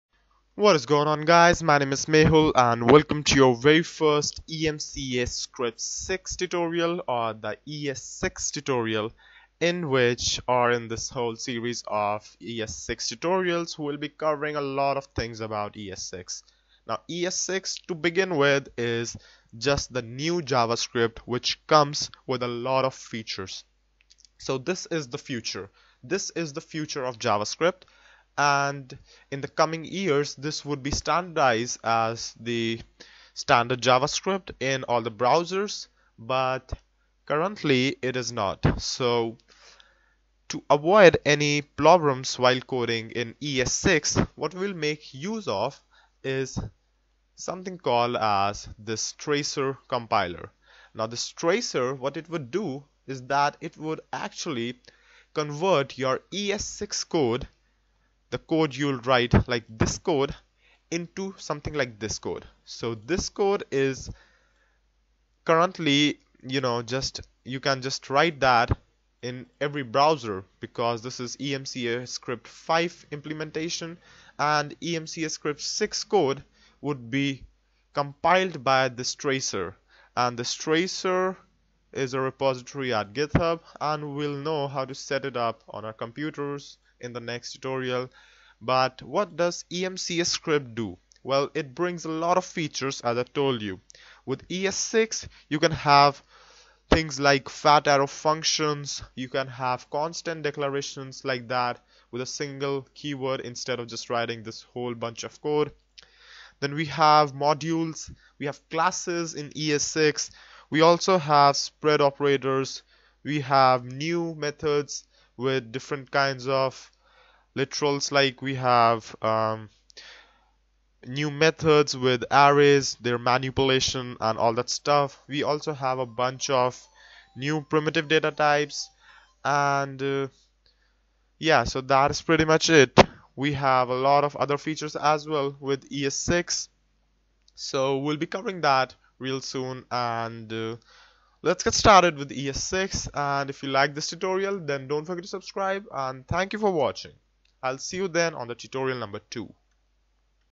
ES6 Tutorial 1: Introduction
Learn what is EMCAScript 6 or ES6 and how to get started with it!